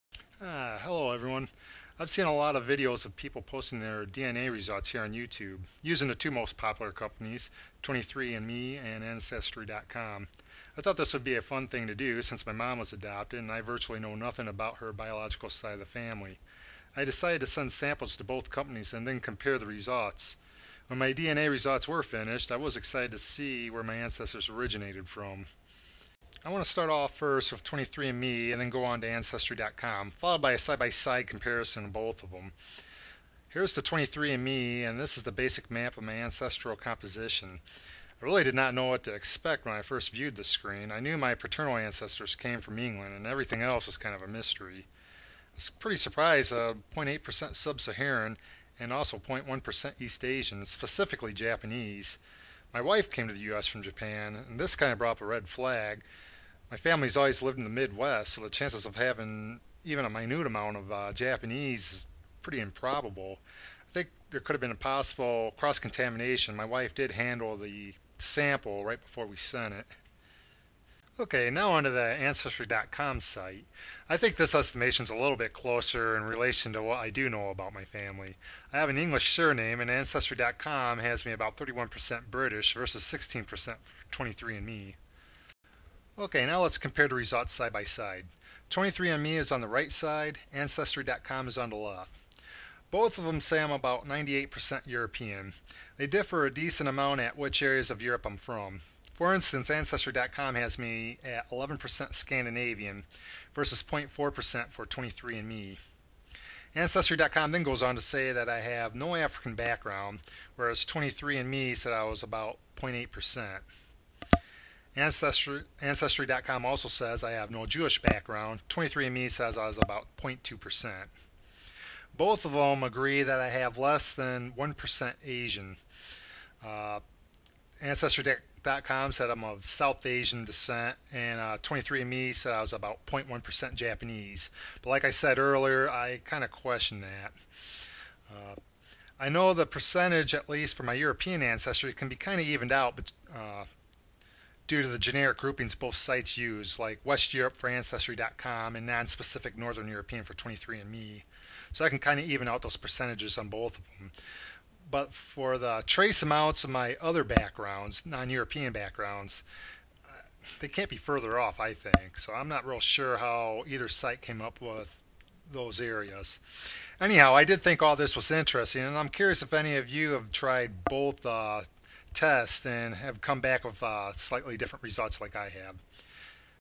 23andme/ancestry.com
I decided to try both test and post the results.